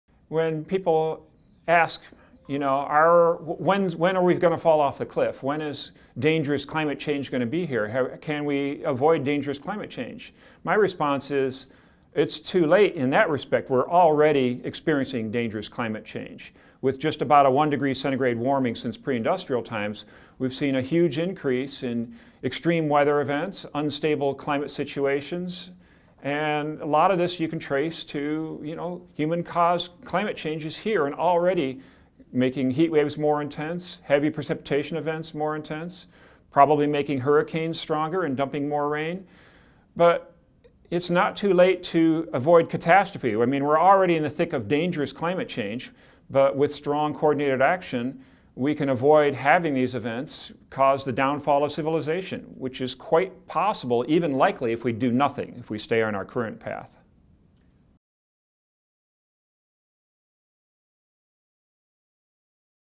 Jeff Masters: Climate Change - How Bad is It?
Weather Underground founder Jeff Masters answers the question - "when do we go over the cliff into dangerous climate change?"